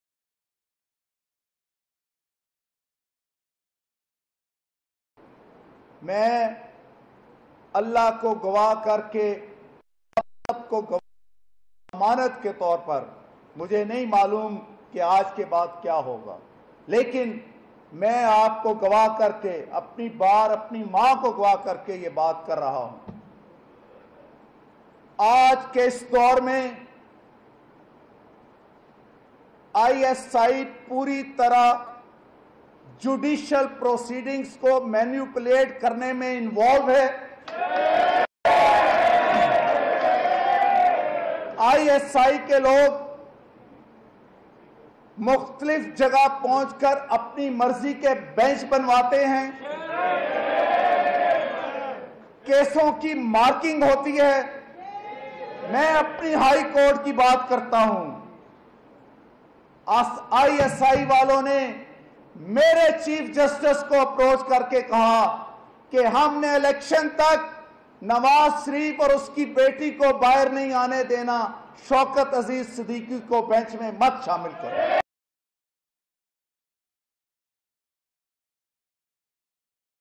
ISI instructed CJ to not release Nawaz Sharif, daughter till elections: Islamabad HC Justice
Islamabad (Pakistan), Jul 22 (ANI): In an address made by Islamabad High Court Justice Shaukat Aziz Siddiqui interference of secret agencies in the state’s affairs were alleged. In his speech, Justice Siddiqui said, “ISI is fully involved in the manipulation of judicial proceedings.